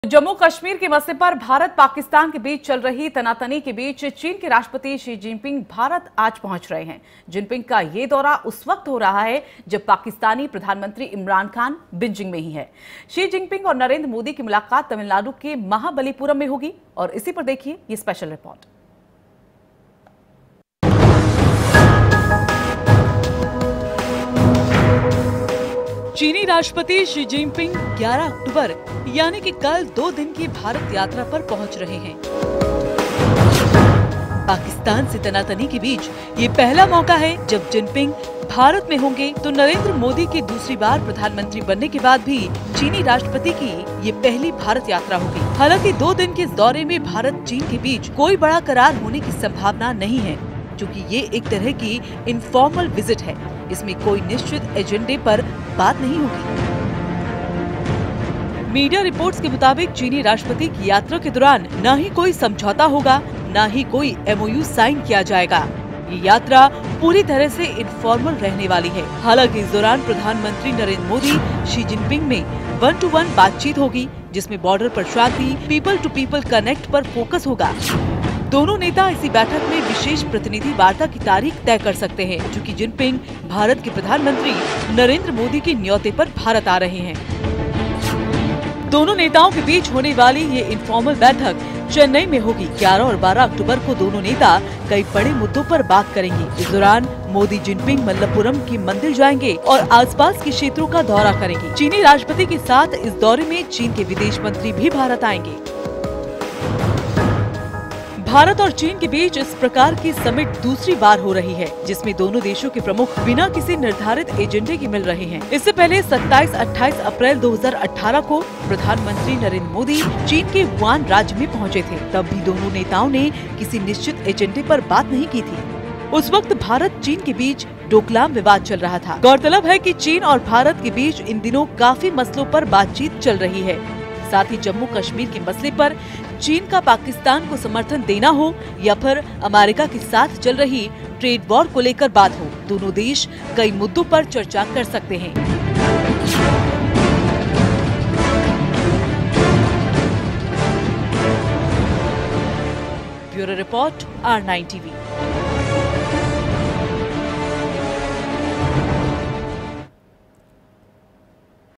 चीन के राष्ट्रपति JINPING के स्वागत के लिए Mamallapuram तैयार
जिनपिंग का ये दौरा उस वक्त हो रहा है जब पाकिस्तानी प्रधानमंत्री इमरान खान बीजिंग में ही हैं.. मामल्लपुरम में मोदी-जिनपिंग की महामुलाकात, कश्मीर पर PAK को मिलेगी मात. जानिए क्या होगा खास हमारी रिपोर्ट में